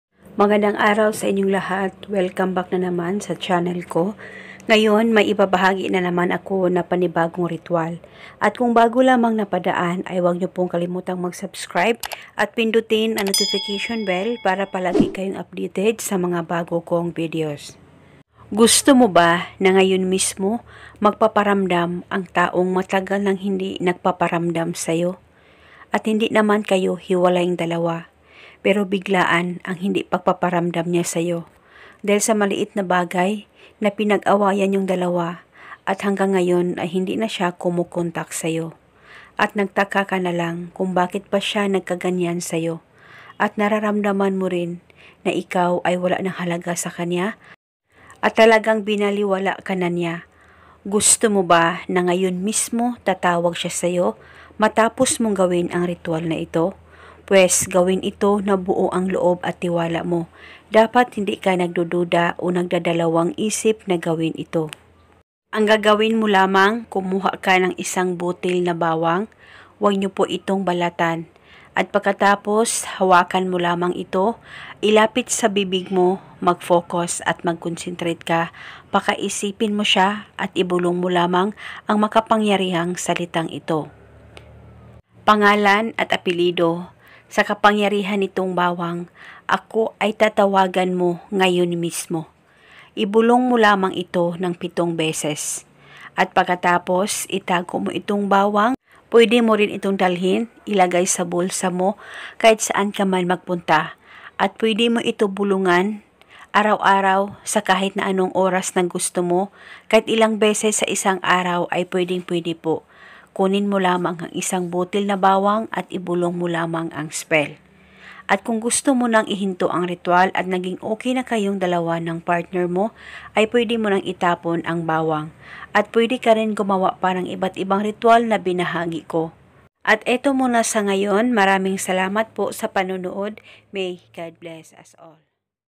KIKILABUTAN KA TATAWAG SIYA SAYO IN JUST 6 MINUTES
gawin mo ang ritwal na ito kung gusto mo na ikaw ay tatawagan niya ngayon mismo